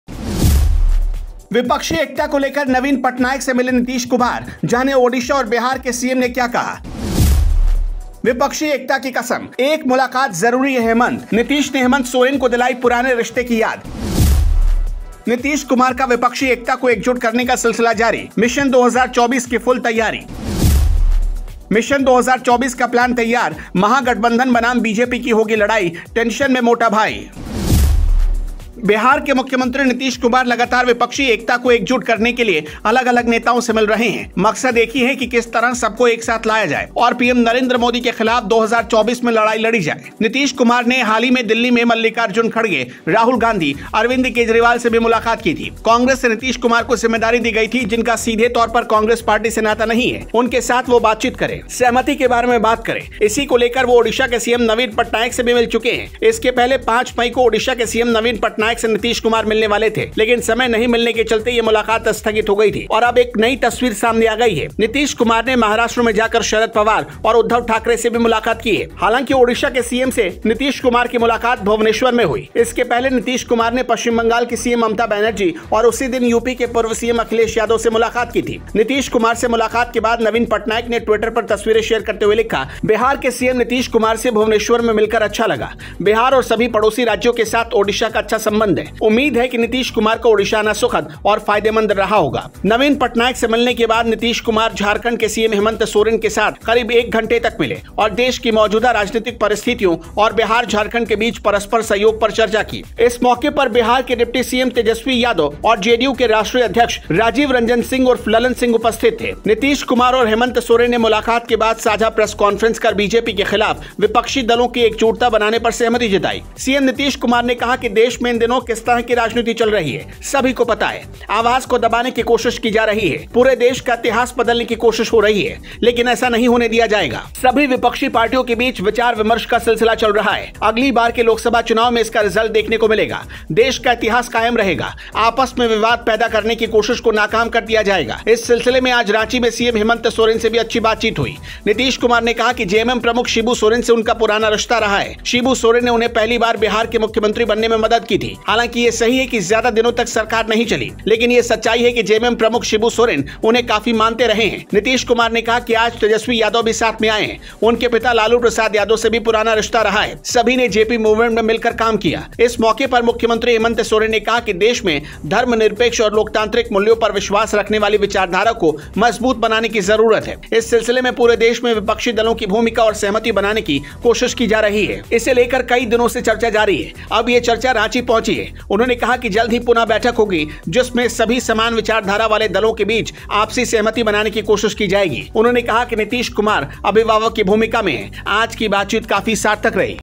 विपक्षी एकता की कसम,एक मुलाकात जरूरी है 'हेमंत',Nitish ने Hemant Soren को दिलाई पुराने रिश्ते की याद
विपक्षी एकता को लेकर नवीन पटनायक से मिले Nitish Kumar , जानें Odisa और बिहार के CM ने क्या कहा, विपक्षी एकता की कसम...एक मुलाकात जरूरी है 'हेमंत', Nitish ने Hemant Soren को दिलाई पुराने रिश्ते की याद